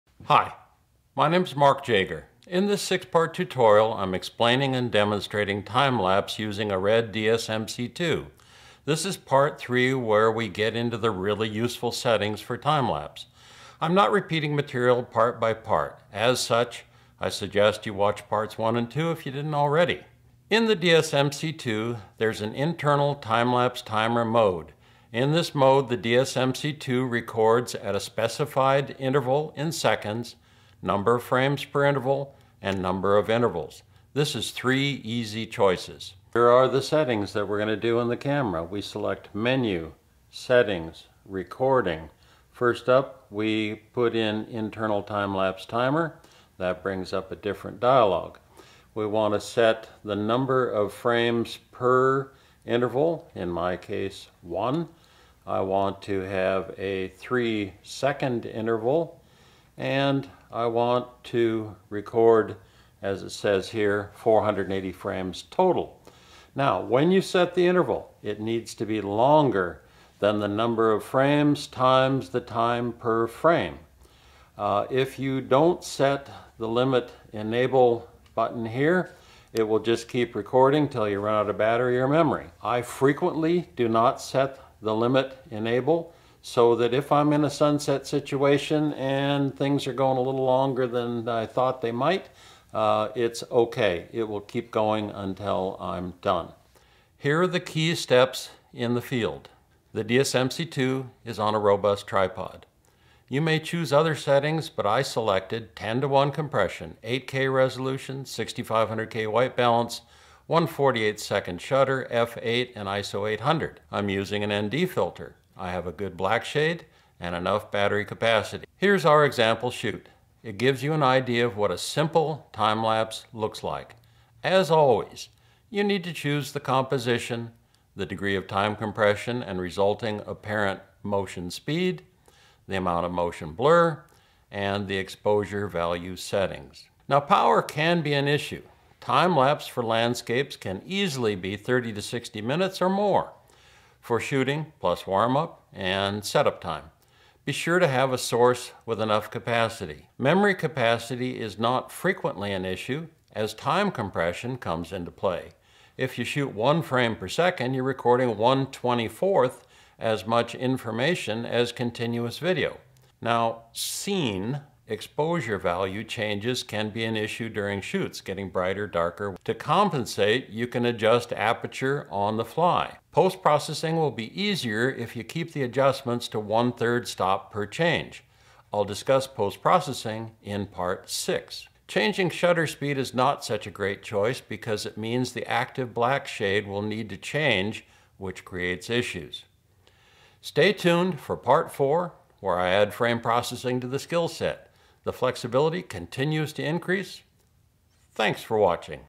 Time-lapse with RED DSMC2 Cameras - Part 3
This is Part 3 of a 6-part tutorial that explains and demonstrates the methods that can easily be used with a Red DSMC2 for time-lapse capture.

In this part, the RED Interval Timelapse Timer Mode is explained.